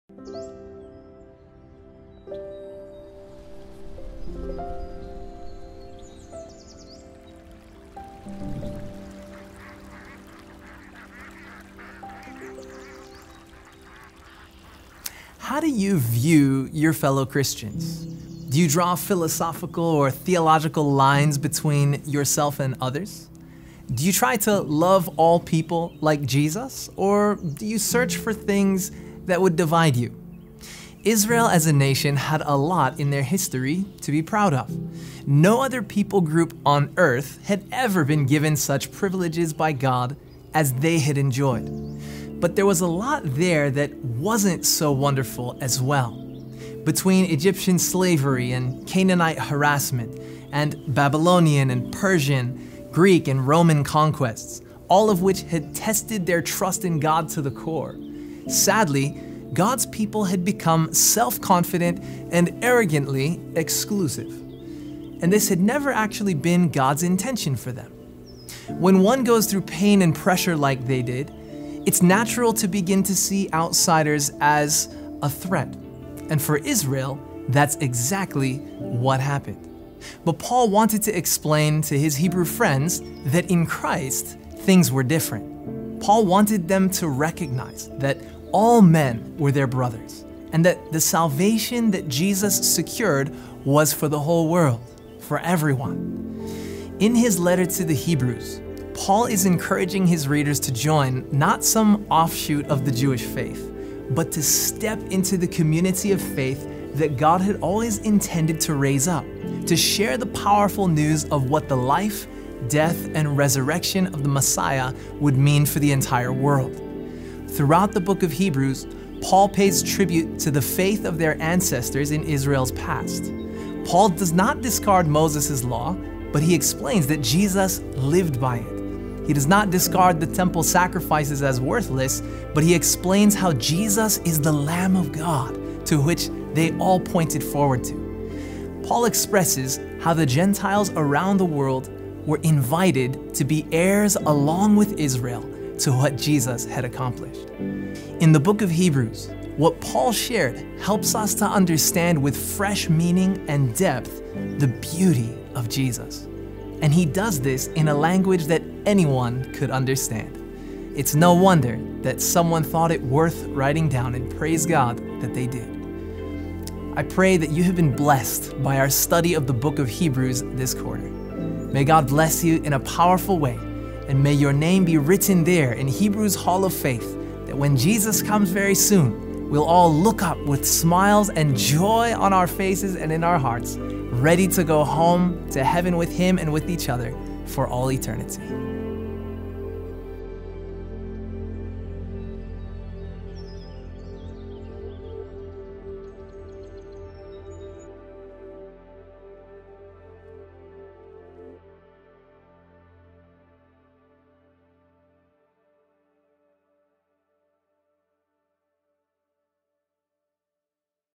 Bible Study Lesson 13 - Let Brotherly Love Continue
Hebrews 13 presents the apostle’s concluding admonition: “Let brotherly love continue” (Heb. 13:1). He has affirmed throughout the epistle that we are of the household of the King-High Priest, Jesus, His brothers and sisters. The author does not conceive of the audience only as a group of individuals who work on their salvation in a one-to-one relationship with Jesus, but as a family, or household, saved together.

2022 Q1: In These Last Days: The Message of Hebrews